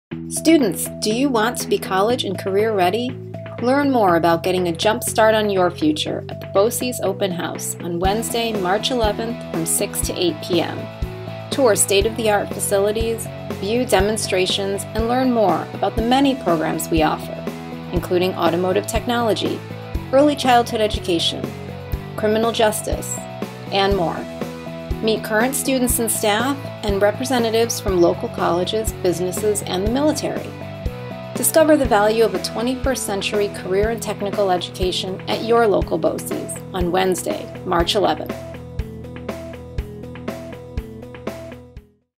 Herkimer BOCES 2020 Open House on March 11
The Herkimer-Fulton-Hamilton-Otsego BOCES 2020 Open House takes place on Wednesday, March 11, at 352 Gros Blvd. in Herkimer.